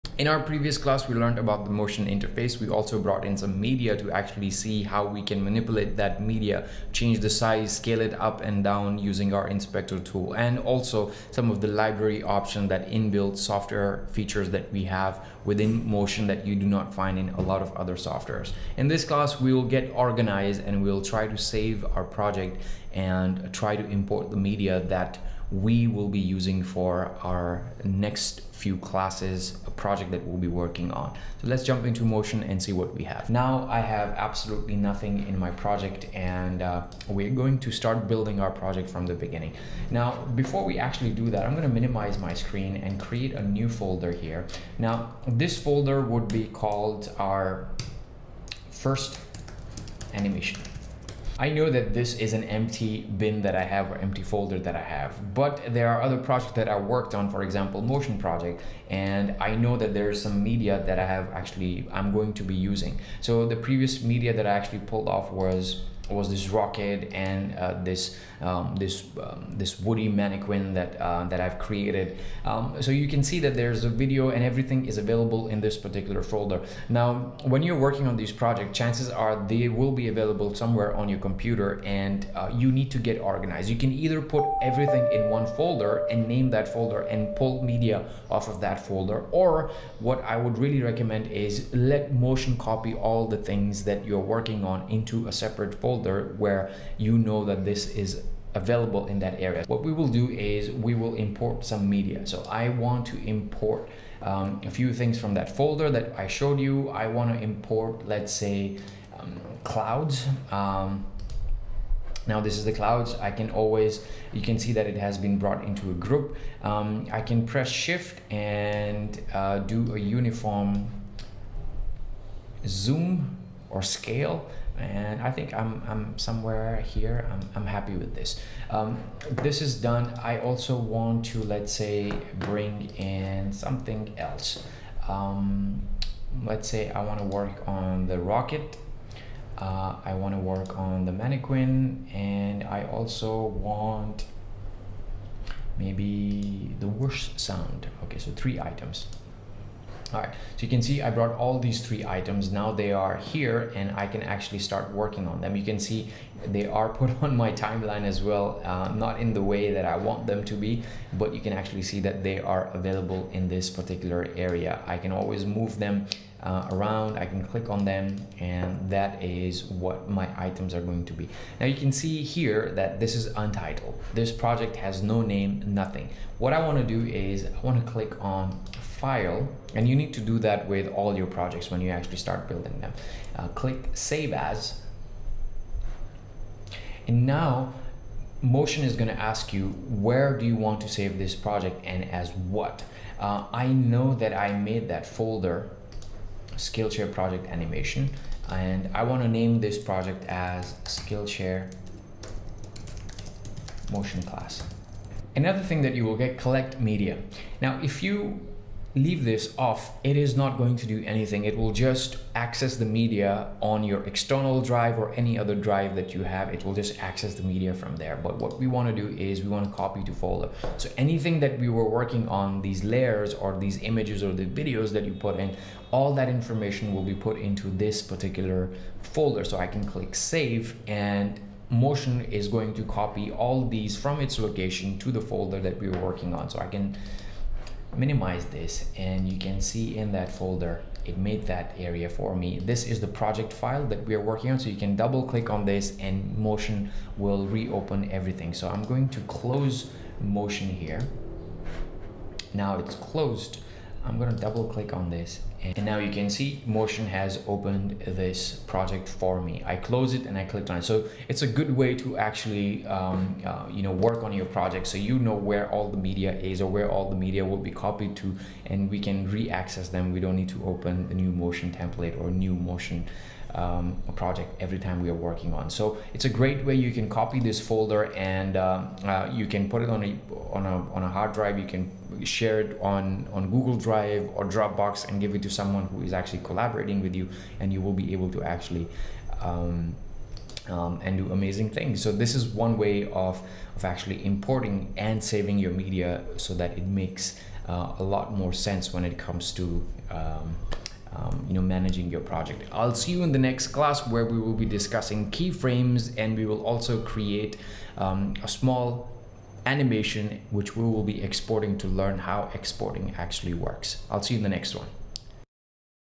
Importing Made Easy: A Beginner's Guide to Streamlining Your Workflow in Apple Motion
Bring your vision to life with ease using Apple Motion's powerful importing tools. This beginner-friendly course will teach you everything you need to know about importing assets into your animation projects. From media files to graphic elements, you'll learn how to organize and streamline your workflow for maximum efficiency. With clear explanations and hands-on demonstrations, you'll be able to master the art of importing in no time. Whether you're a filmmaker, graphic designer, or just looking to expand your skills, this comprehensive guide to importing in Apple Motion is perfect for anyone who wants to streamline their workflow and bring their ideas to life.